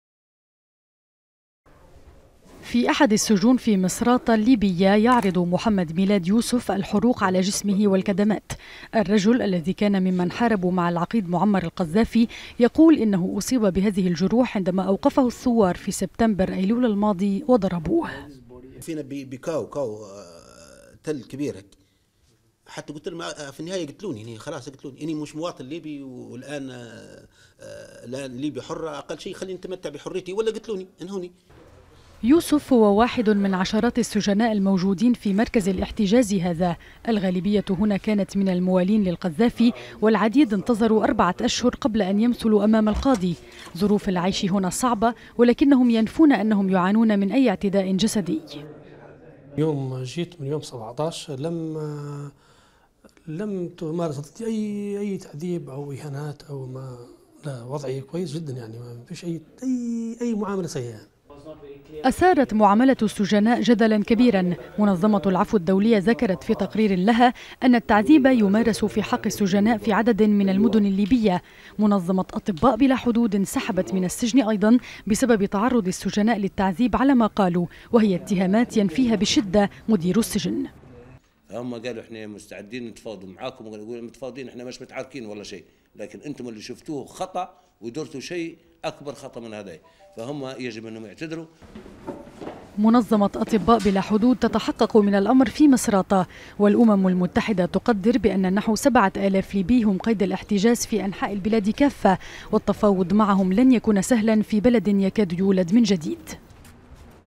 ممارسة التعذيب في حق السجناء الموالين للقذافي
الأوضاع في السجون الليبية كانت من المواضيع الأكثر جدلا في ليبيا، بعد أن علقت منظمة أطباء بلا حدود عملها في مصراتة، قائلة بأن أطبائها واجهوا مرضى كانوا يعانون من اصابة ناجمة جراء التعذيب الذي تعرضوا له، خلال استجوابهم. تقرير مسجل. محمد حداد/ نشوة ناصرالدين/ غراسيا بيطار